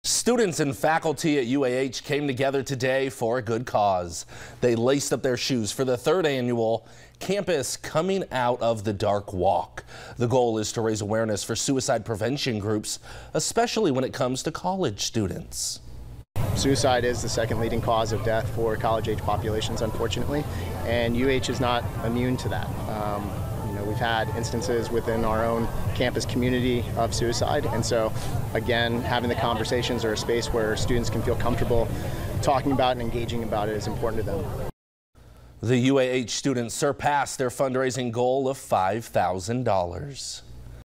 UAH holds campus walk for suicide prevention and awareness
Student volunteers at the University of Alabama Huntsville along with the American Foundation for Suicide Prevention held its third annual walk